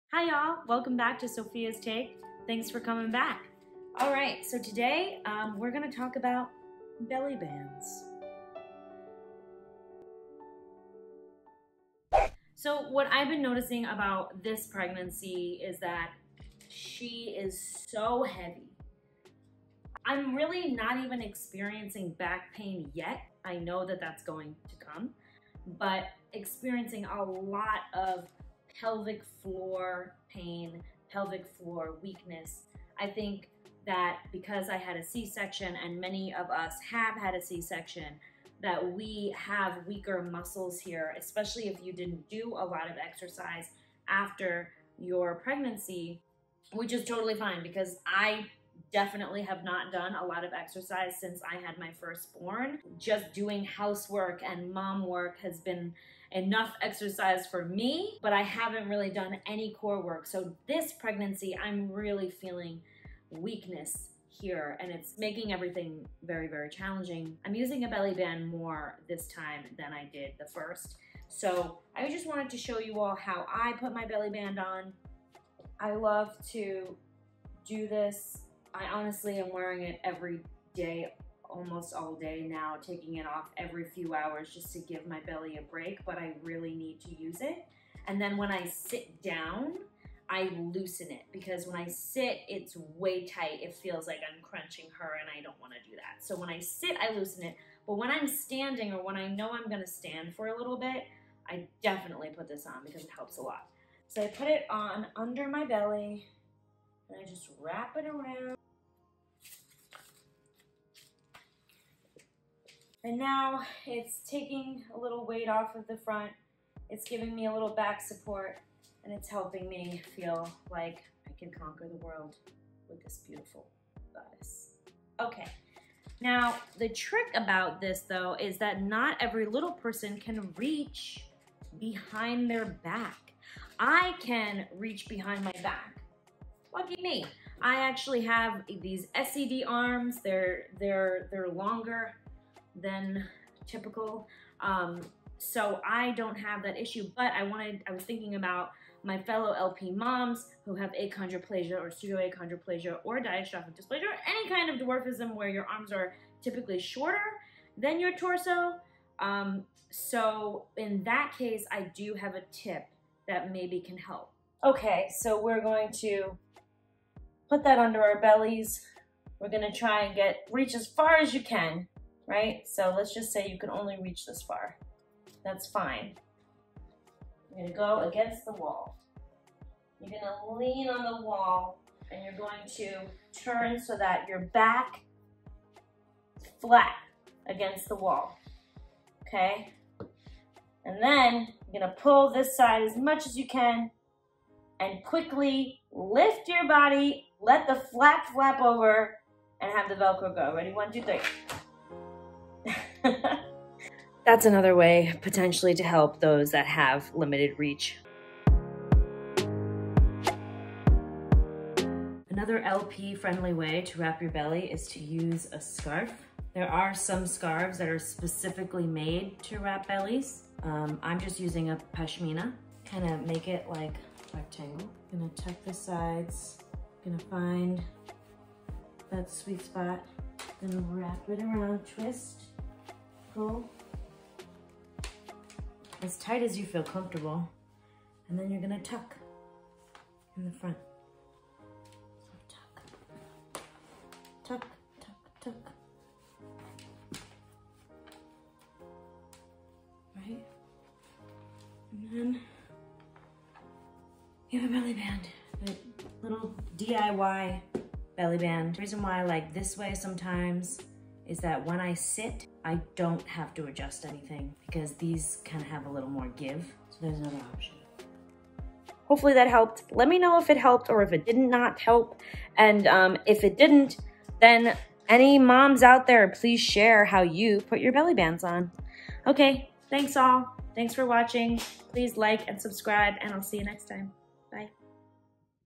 Belly Band TIPS for moms with dwarfism
Belly Bands are saving me this pregnancy! But I know this might not be the easiest option for LP moms. I share a few tips in this video. Let me know what’s your favorite bellyband that you use in the comments section below .

AZ Med Belly Band

Pregnancy Yoga Shorts